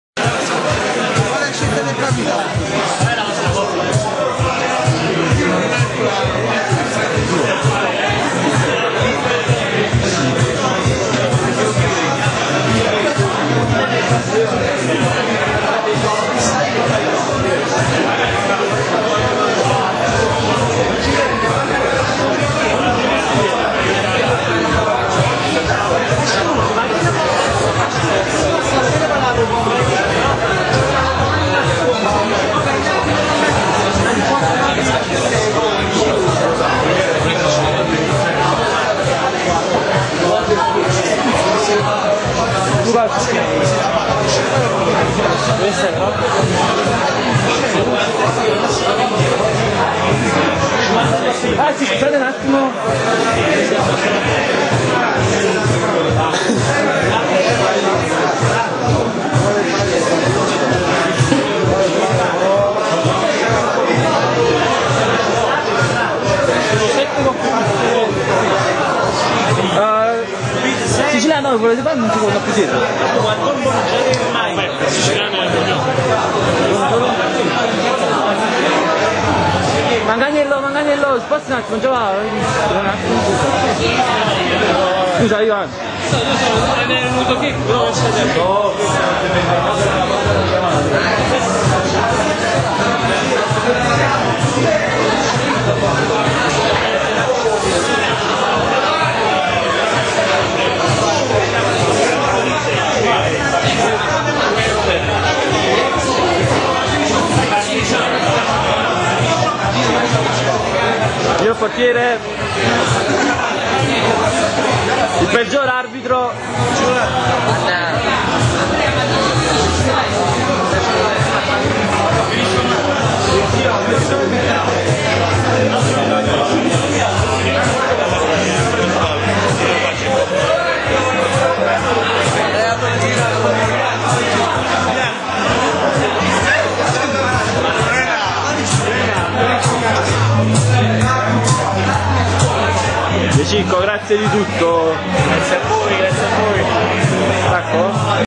PREMIAZIONE TORNEO PREMIER LEAGUE 2009-10 ELASTIC BOYS CLUB.wmv
Il video della premiazione del XXV Torneo di calcio a 5 " Premier League 2009/10" svolto all'Elastic Boys Club di Napoli.